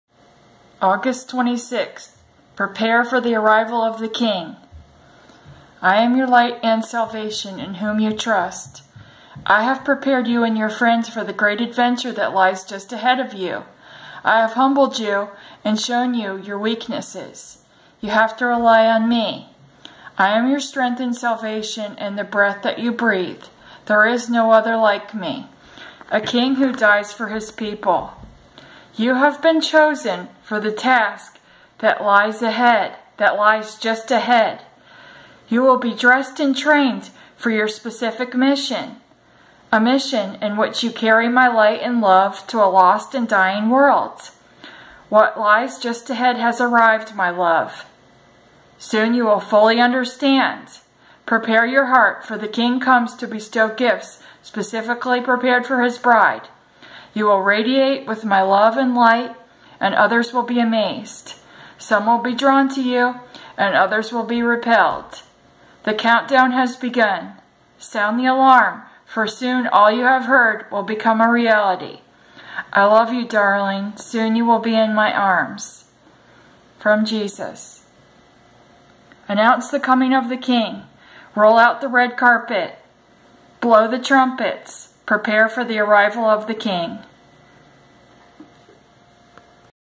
Prepare for the Arrival of the King/ You have been Chosen
8/26/2018

I am your light and salvation in whom you trust. I have prepared you and your friends for the great adventure that lies just ahead for you. I have humbled you and shown you your weaknesses. You have to rely on Me. I am your strength and salvation and the breath that you breathe. There is no other like Me. A King who dies for His people. You have been chosen for the task that lies just ahead. You will be dressed and trained for your specific mission. A mission in which you carry My Light and Love to a lost and dying world. What lies just ahead has arrived, My Love. Soon you will fully understand. Prepare your heart for the King comes to bestow gifts specifically prepared for His Bride. You will radiate with My love and light and others will be amazed. Some will be drawn to you and others will be repelled. The countdown has begun. Sound the alarm for soon all you have heard will become a reality. I love you Darling. Soon you will be in My arms.

Then I saw and heard the next portion:

Announce the coming of the King!.  (I heard this over and over again)

Roll out the red carpet. (I saw this!)

Blow the trumpets. (I heard and saw this!)

Prepare for the arrival of The King! (I saw ticker tape falling and heard a roar of excitement!)

Psalm 27:1
"The LORD is my light and my salvation-- whom shall I fear?"

Isaiah 60:5
"Then you will look and be radiant, and your heart will tremble and rejoice, because the riches of the sea will be brought to you, and the wealth of the nations will come to you."

Isaiah 62:11
"Behold, the LORD has proclaimed to the ends of the earth, "Say to Daughter Zion: See, your Savior comes! Look, His reward is with Him, and His recompense goes before Him."

Acts 13:47
"For this is what the Lord has commanded us: “‘I have made you a light for the Gentiles, that you may bring salvation to the ends of the earth."